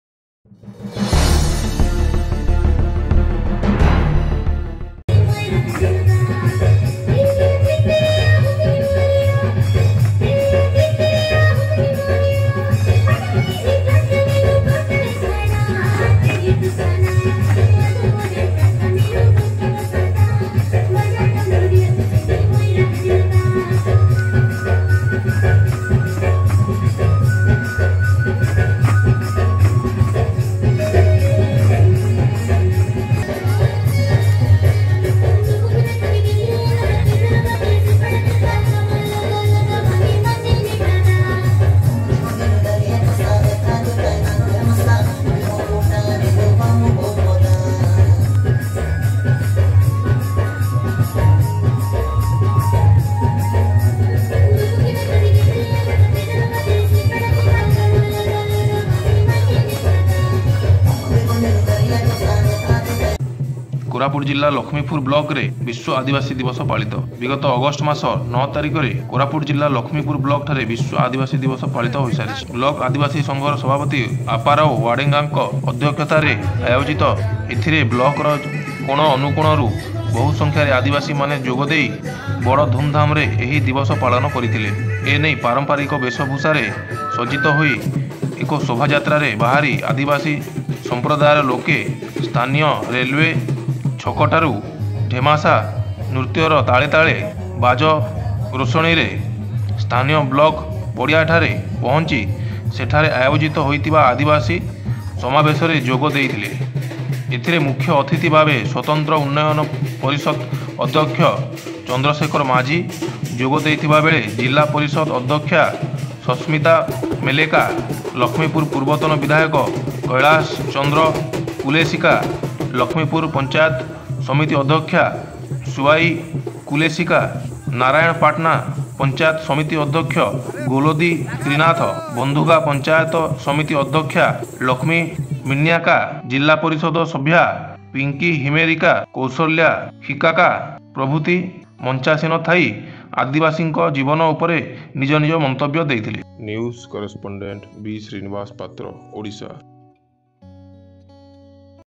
ପାରମ୍ପରିକ ଆଦିବାସୀ ନୃତ୍ୟ ଓ ବାଦ୍ୟ ରେ ଦୁଲୁକିଲା ସାରା ଲଷ୍ମୀପୂର କୋରାପୁଟ | SangharLive
ପାରମ୍ପରିକ ଆଦିବାସୀ ନୃତ୍ୟ ଓ ବାଦ୍ୟ ରେ ଦୁଲୁକିଆ ସାରା ଲଷ୍ମୀପୂର କୋରାପୁଟ | SangharLive